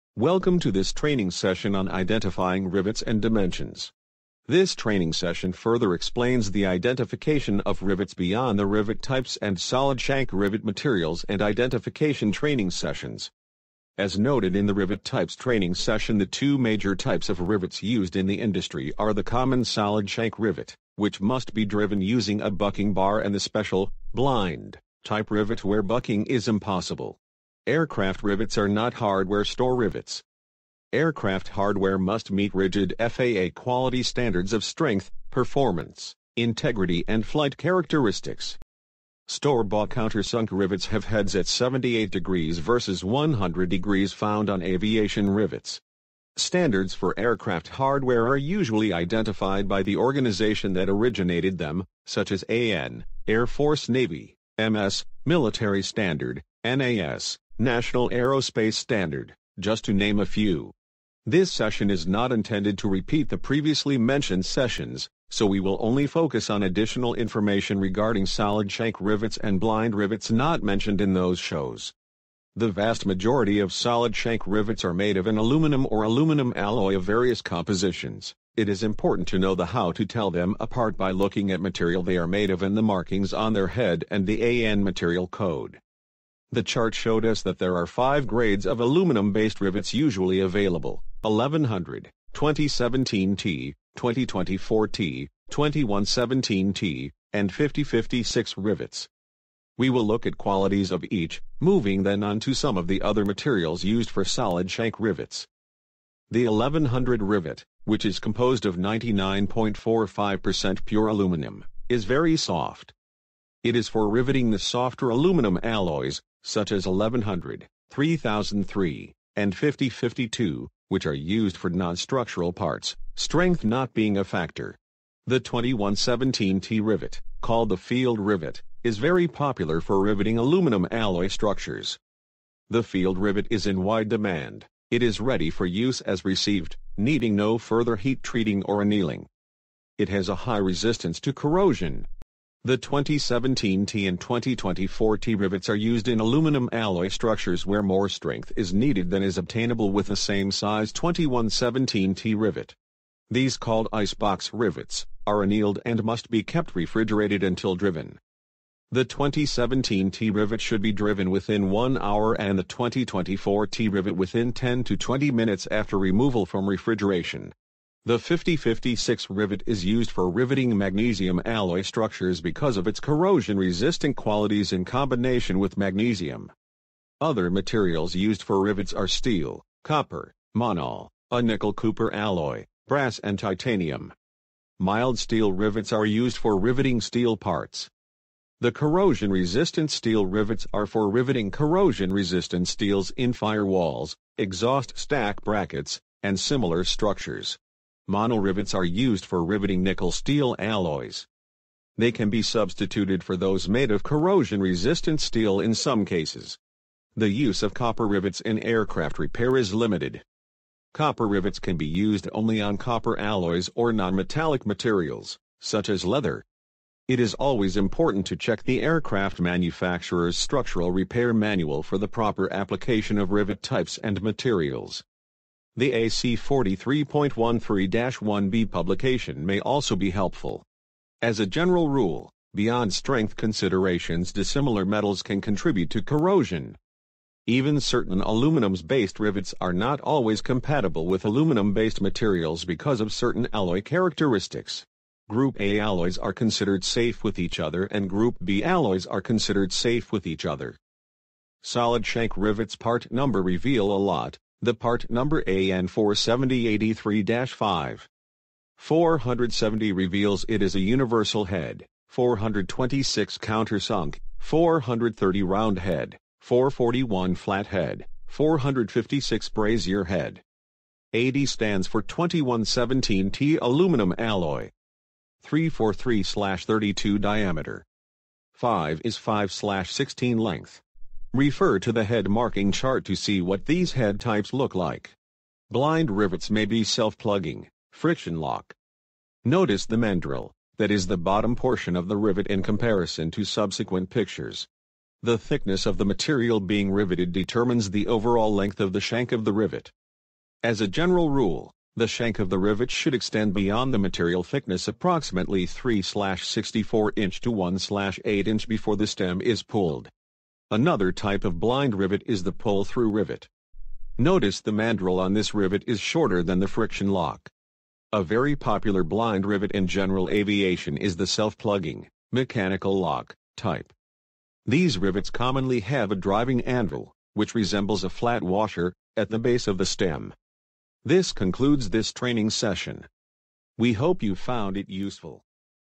The Ultimate Guide to Identifying Rivets and Dimensions
Rivets are an essential component in a wide range of applications, from construction and engineering to aerospace and automotive industries. Understanding the types of rivets available and their dimensions is crucial for selecting the right one for your project.

In this video, we'll take you through the basics of rivets, starting with an overview of the different types, including blind rivets, solid rivets, and tubular rivets. We'll also discuss the pros and cons of each type and their ideal applications.

Next, we'll dive into the nitty-gritty details of rivet dimensions, such as head diameter, grip range, and overall length. We'll teach you how to measure these dimensions accurately, and what factors to consider when selecting the right size and shape of rivet.

To make things even more interesting, we'll provide real-life examples of how rivets are used in various industries, from the construction of aircraft to automotive assembly lines. You'll be amazed at the diverse applications of this small but mighty component.

So, whether you're a DIY enthusiast or a professional engineer, this video is the perfect place to start mastering the art of identifying rivets and understanding their dimensions.